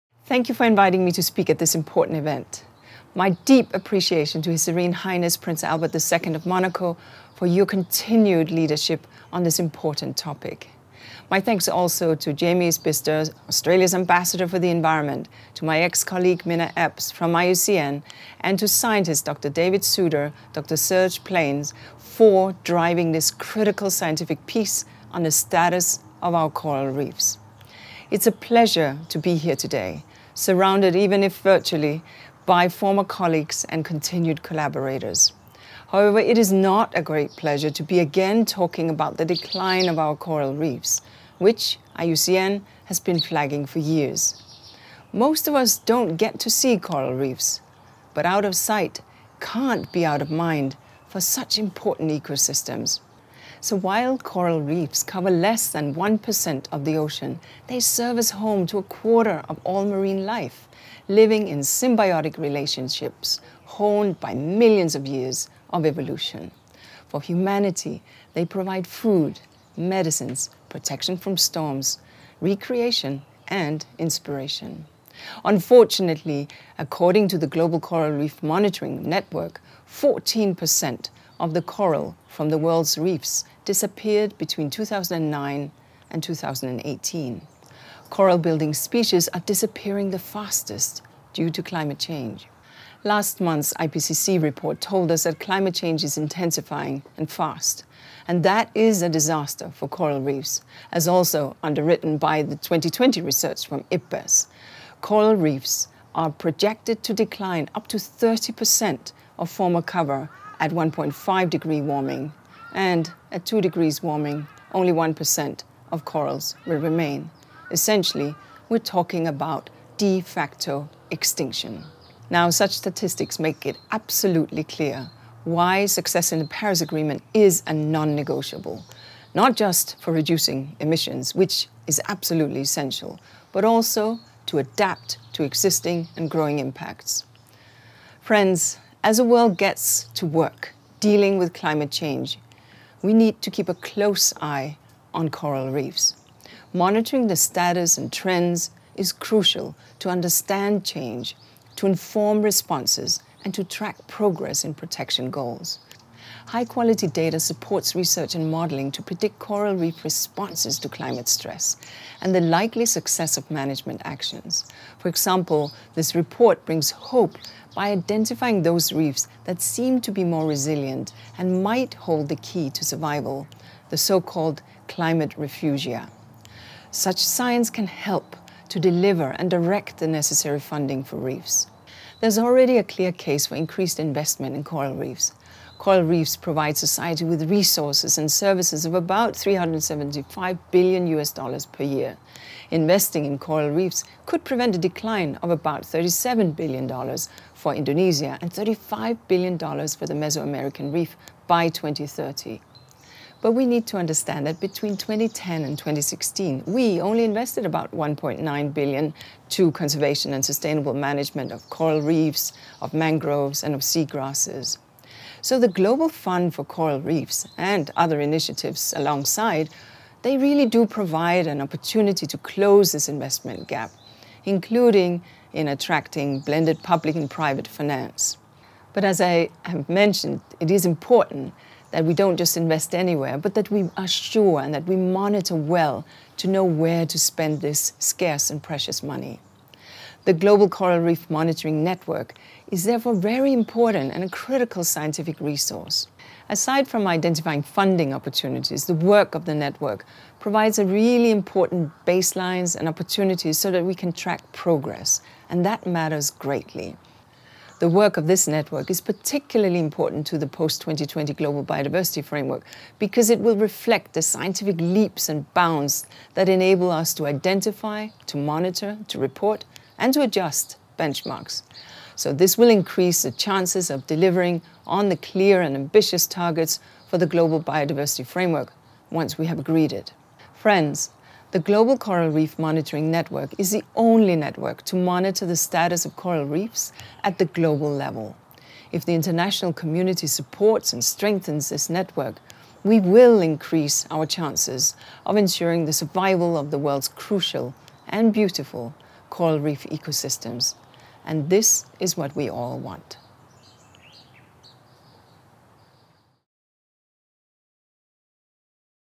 The Importance of Monitoring in the Context of the UN Decades - Inger Anderson
Inger Anderson, Executive Director, UNEP, address during the "Sixth Status of Coral Reefs of the World: 2020" event for the Global Coral Reef Monitoring Network (GCRMN) by the International Coral Reef Initiative.

IUCN World Conservation Congress - 7th September, 2021, 11:15 - 12:00.